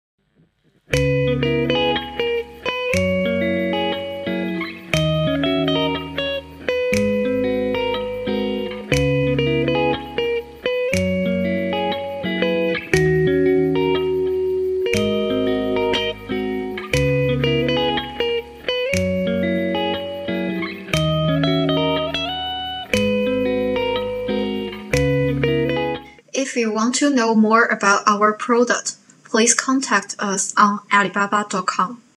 Hitachi ZX120 crawler excavator appearance show around
Used ZX120 95% new 360 degrees appearance show around, medium hydraulic backhoe crawler excavator, good performance in mini and municipal construction works